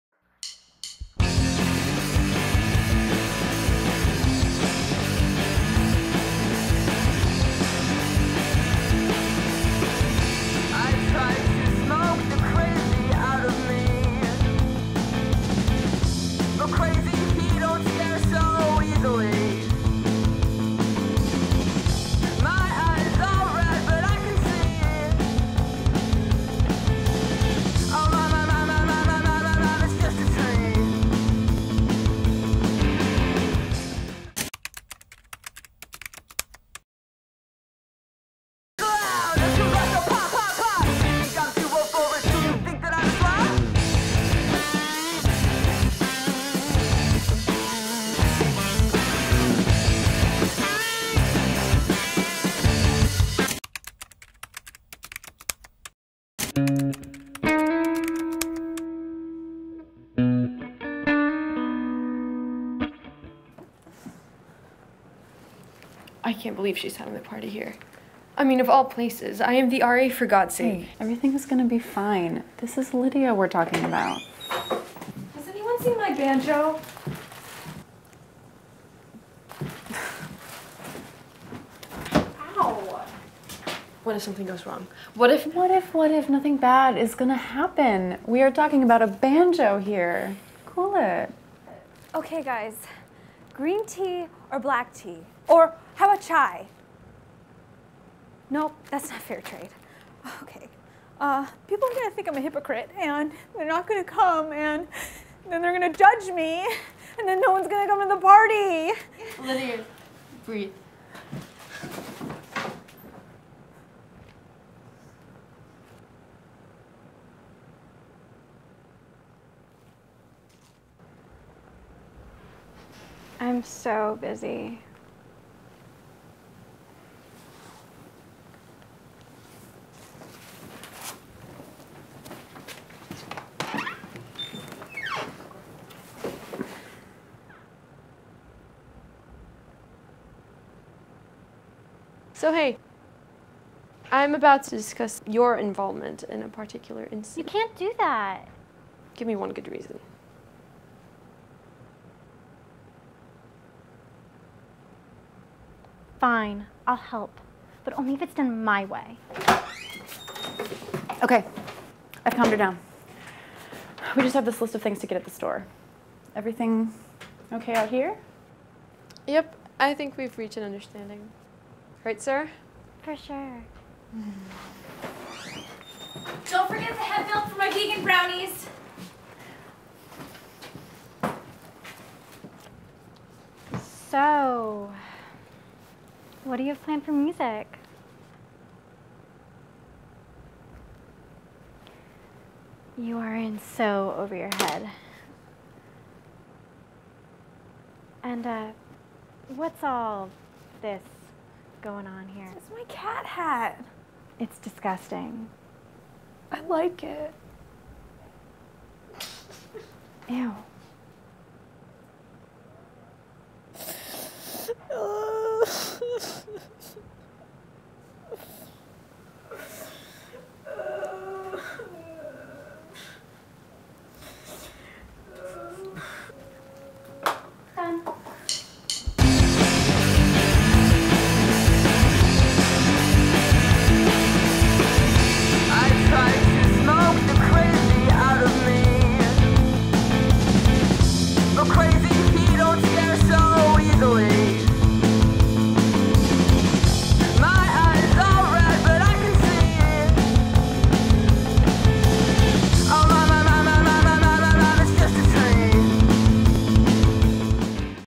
"Socially Active" Episode #5
A collaborative webseries created by Theatre and Filmmaking & Media Arts Students, Faculty, Staff and Alumnae at Sarah Lawrence College.